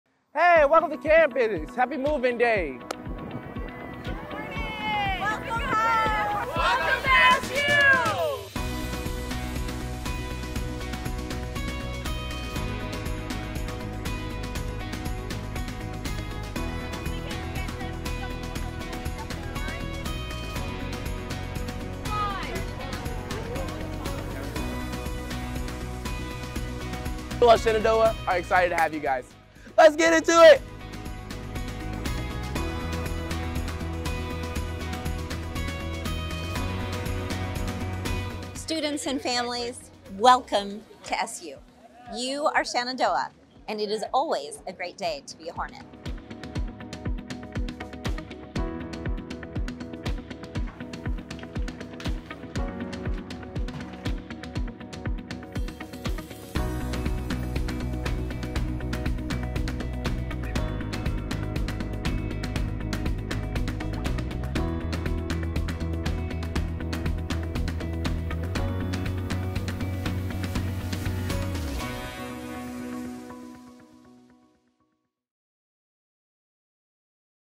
Move-In Day 2023
We were so excited to welcome hundreds of new students to campus! We cannot wait to see what they accomplish!